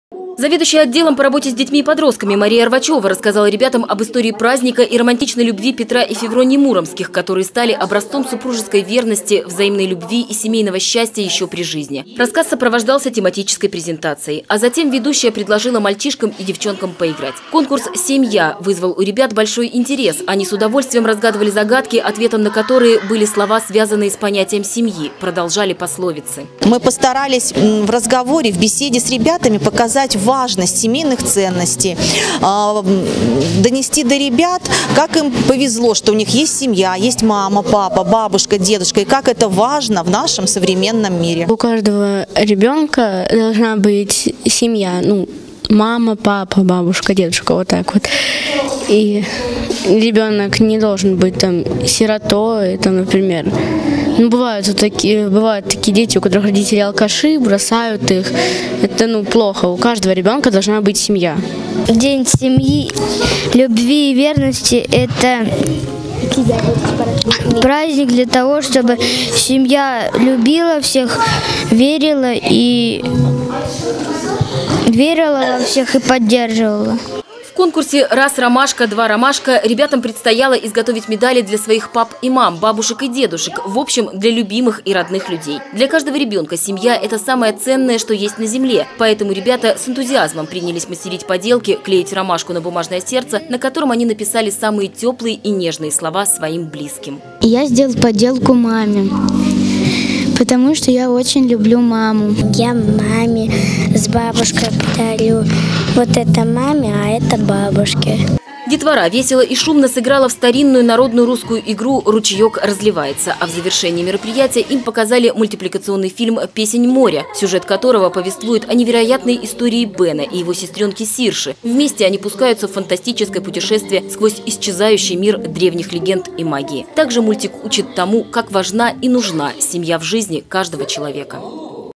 Десна-ТВ: Детям из лагеря рассказали о празднике День семьи, любви и верности
В Городском центре культуры и молодёжной политики прошла игровая программа, посвящённая Дню семьи, любви и верности «Подари ромашку» для детей из летнего лагеря «Улыбка» школы №3. Подробности смотрите в нашем сюжете.